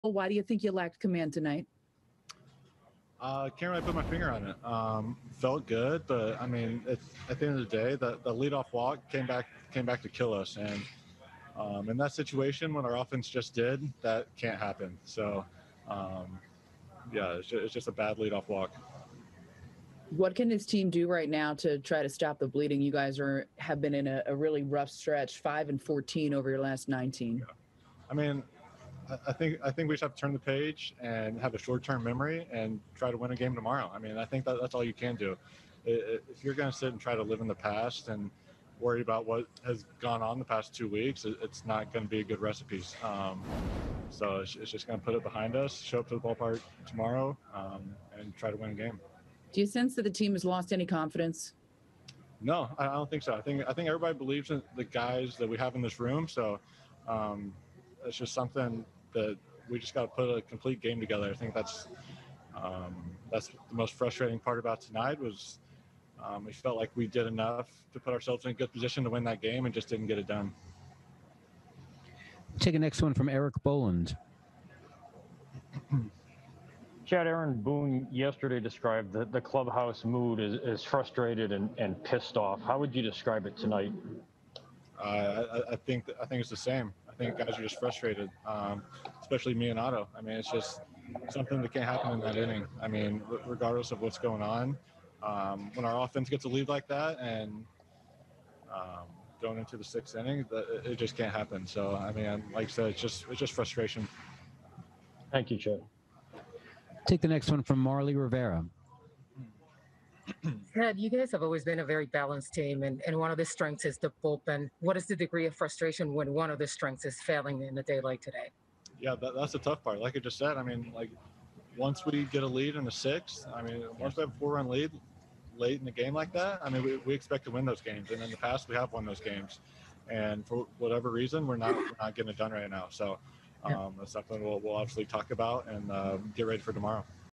Chad Green discusses struggles vs. Blue Jays
Chad Green discusses his tough night on the mound after a 12-7 loss to the Blue Jays on Monday.